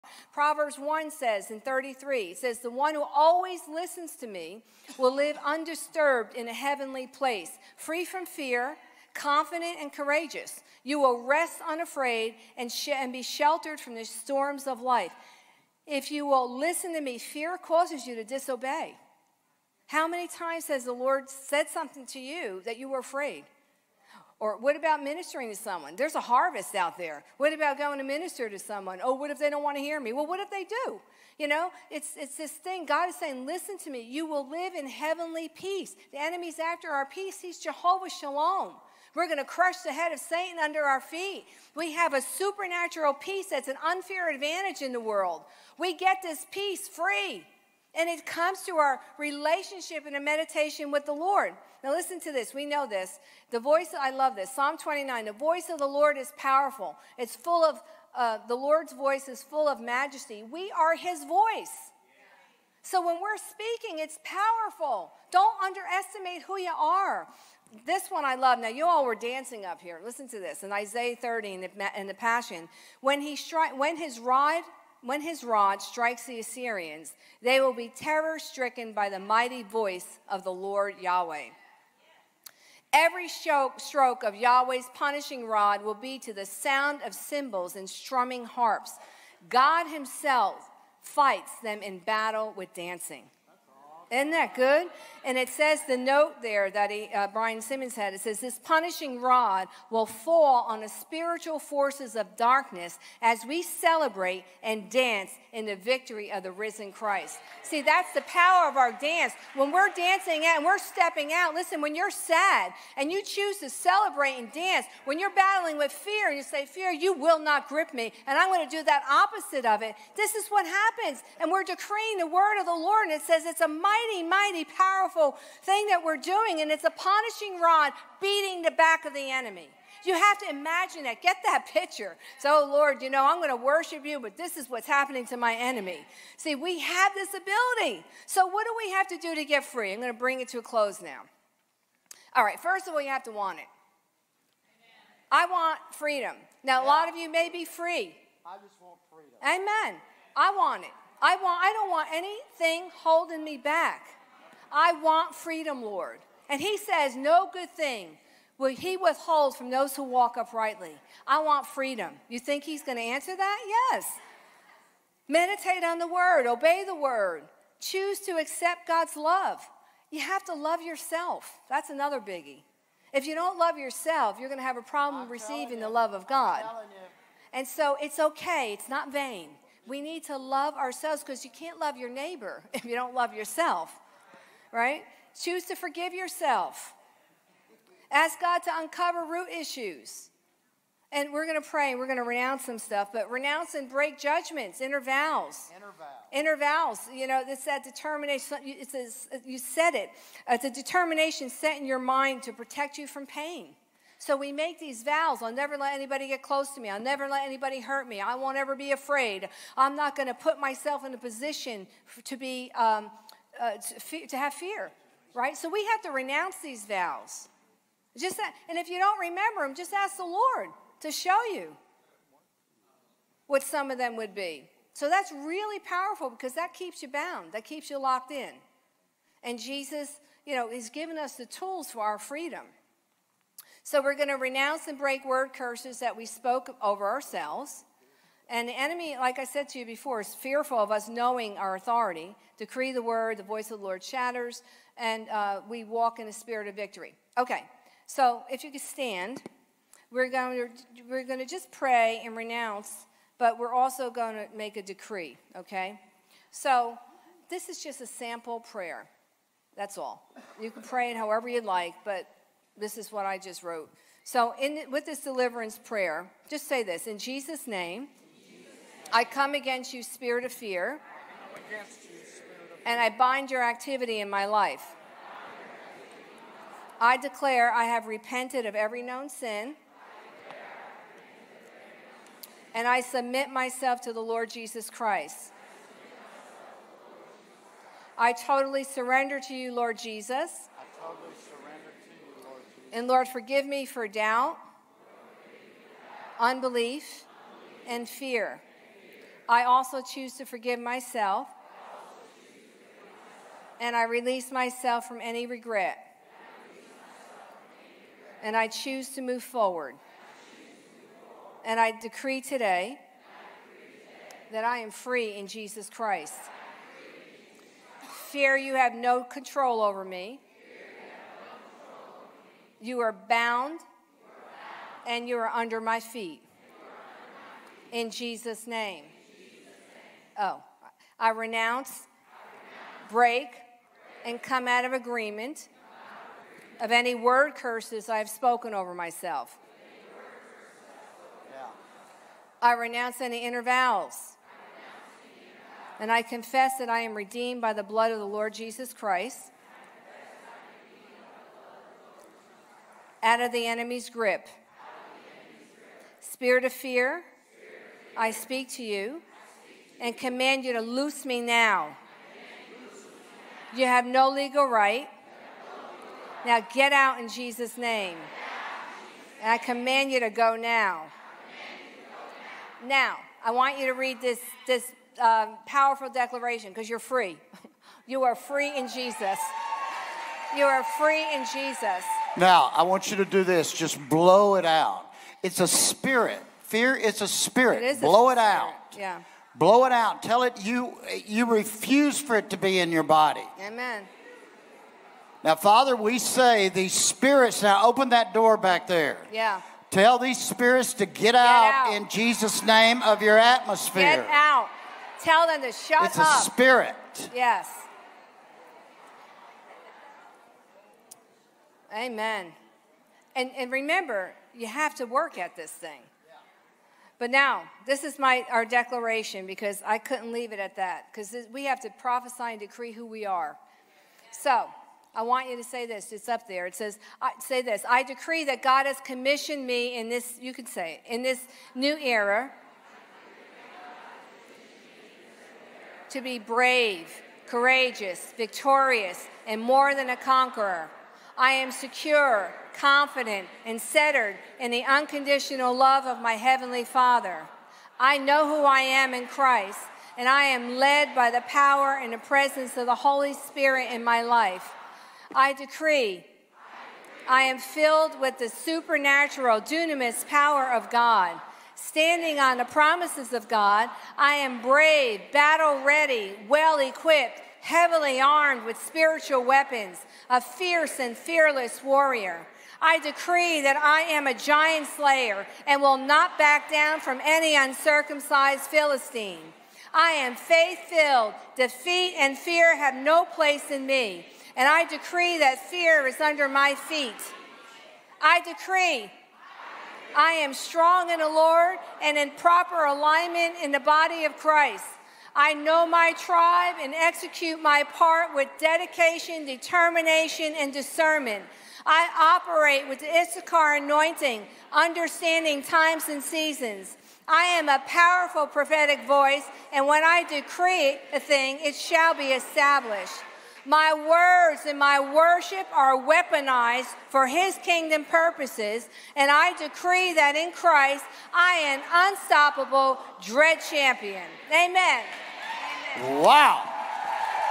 Trisha Roselle at Glory of Zion: The Lord is With Me Like a Dread Champion! (Jeremiah 20:11)
Trisha speaks at Glory of Zion in February 2020. Jeremiah 20:11 says, “But the LORD is with me like a dread champion; therefore, my persecutors will stumble and not prevail. They will be utterly ashamed, because they have failed, with an everlasting disgrace that will not be forgotten.” Trisha has been helping people find freedom from addictions, ungodly beliefs, counterfeit affections, destructive relationships and other forms of oppression for decades. She and her husband Peter began King of Kings Worship Center in Basking Ridge, NJ in 1999, a vibrant body of believers committed to knowing Christ and making Him known in the fullness of His power.

Be blessed.

This video published with permission from Chuck Pierce.

I decree that God has commissioned me in this
new era to be brave, courageous, victorious,
and more than a conqueror.
I am secure, confident, and centered
in the unconditional love of my Heavenly Father.
I know who I am in Christ and I am led by the Power
and Presence of Holy Spirit in my life!
I decree I am filled with the supernatural, dunamis power of God.
Standing on the promises of God, I am brave,
battle-ready, well-equipped, and heavily armed with
spiritual weapons.
I am faith-filled, fierce and fearless warrior!
I decree that I am a giant-slayer and will not
back down from any uncircumcised Philistine!
Defeat and fear have no place in me!
I decree that FEAR IS UNDER MY FEET!
I decree that I am strong in the Lord and in proper
alignment in the Body of Christ.
I know my tribe and execute my part with
dedication, determination, and discernment!
I operate with the Issachar anointing,
understanding times and seasons!!!
I am a powerful prophetic voice and
when I decree thing, it shall be established!!!
My words and my worship are weaponized
for His Kingdom purposes!
I decree that in Christ,
I am an UNSTOPPABLE
DREAD CHAMPION!!!
AMEN!

Fear will cause you to  surrender your authority!
Luke 4:6
And the devil said to Him,
“All this authority I will give You, and their glory;
for this has been delivered to me,
and I give it to whomever I wish.

Jesus Christ took the authority back.
Satan got it from Adam and Eve, but I want you to see this.
Satan said, “All this authority I will give you.”
Well, we have all this authority now!

Matthew 10 (AMP)
1 Jesus summoned His twelve disciples
and gave them authority and power
over unclean spirits, to cast them out,
and to heal every kind of disease and every kind of sickness.

Proverbs 1 (TPT)
33 But the one who always listens to me
will live undisturbed in a heavenly peace.
Free from fear, confident and courageous,
you will rest unafraid and sheltered from the storms of life.

Isaiah 30 (TPT)
31 And when his rod strikes the Assyrians, they will be
terror-stricken by the mighty voice of Lord Yahweh.
32 Every stroke of Yahweh’s punishing rod will be
to the sound of cymbals and strumming harps.
God Himself fights them in battle with dancing!

Note from TPT: “This “punishing rod” will fall on the spiritual
forces of darkness as we celebrate and dance in the victory of the risen Christ.”

How to get free:
You must desire it; mediate on the word
Obey the word; decide to accept God’s love
Choose to love yourself;
Choose to forgive yourself
Ask God to uncover root issues;
Renounce and break judgements, and vows such as,
“I will never let anyone get close to me.”
 Do not submit to the fear of being rejected.
Renounce and break word-curses you have spoken over yourself.
Enemy is fearful of us knowing and using our authority.
Decree the Word of the Lord – the Voice of the Lord shatters the enemy.
Spirit of victory - we can walk in victory because of God’s Spirit within us.

Deliverance prayer:
In Jesus Name, I come against you spirit of fear and I bind your activity in my life.
I declare I have repented of every known sin,
I submit my self to the Lord Jesus Christ.
I totally surrender to you Lord Jesus.
Lord forgive me for doubt, unbelief and fear.
I also choose to forgive myself, release myself from any regret.
I choose to move forward.
I renounce, break  and come out of agreement
of any word curses I have spoken over myself.
I renounce any inner vow I confess that I am
redeemed by the Blood of the Lord Jesus Christ
out of the enemy’s grip.
Spirit of fear, I speak t you
and command you to loose me.
You have NO LEGAL RIGHT – NOW GET OUT!
I command you to leave me now in Jesus Name!